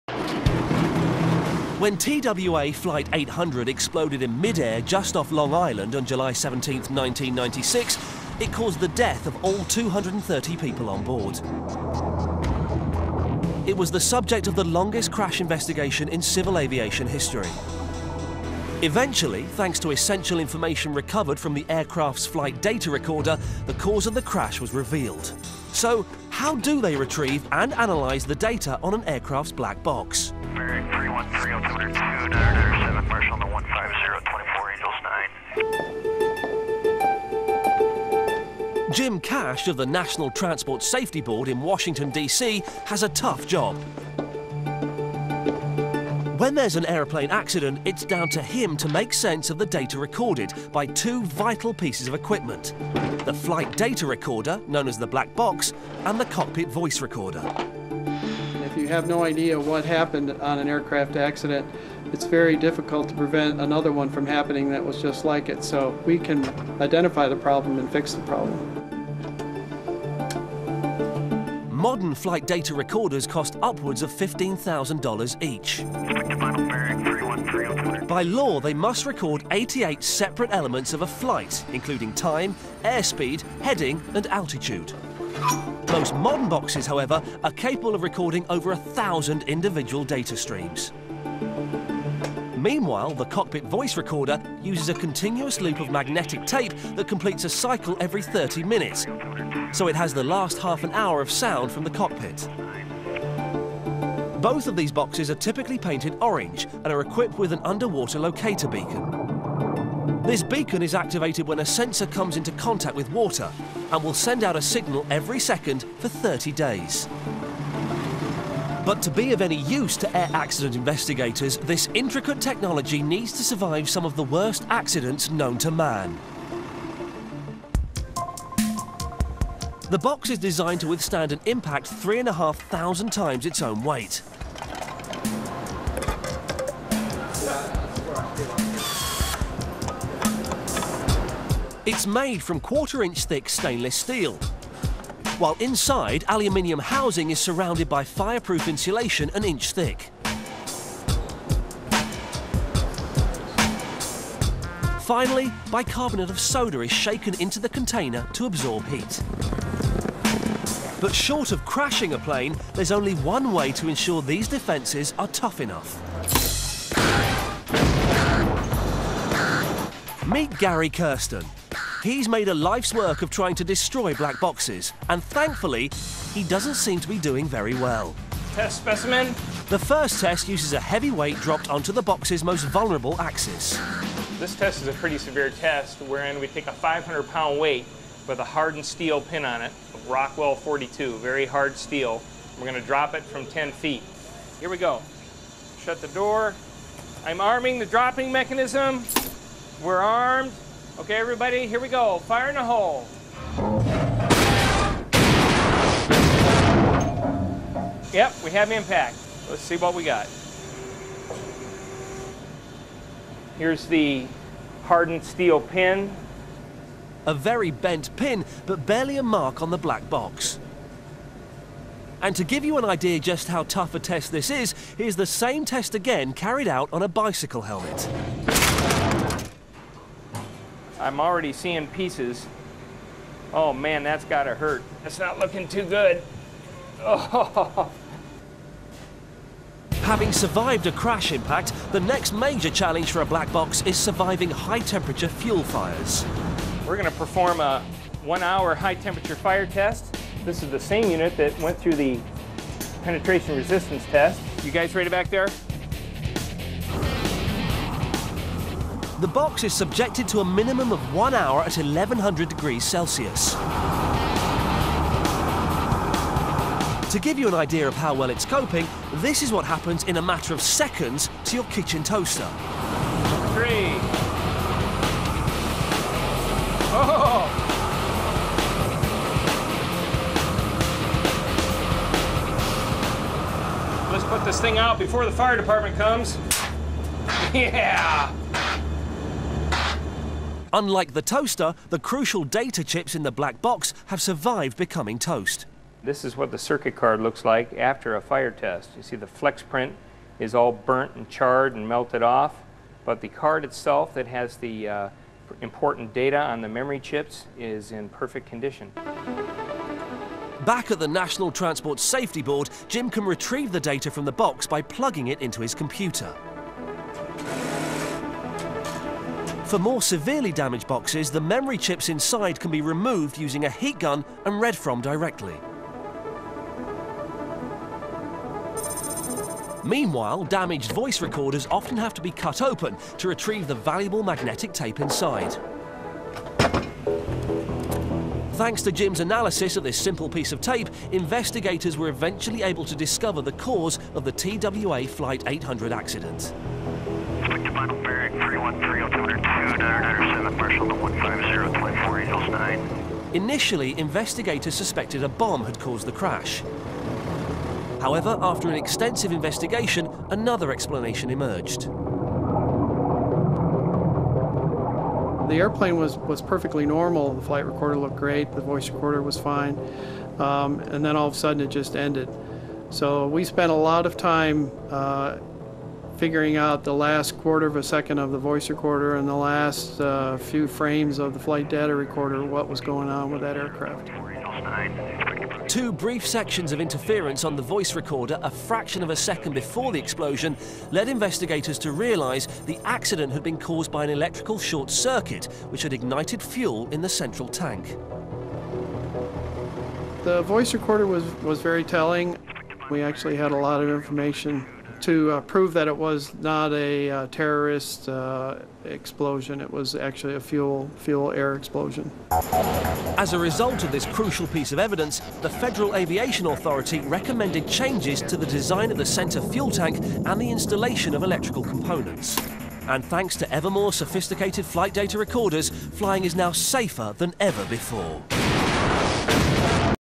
Flight Box Recorders : Retrieving and Analysing Data
How flight box recorders are made and the process of retrieving and analysing data.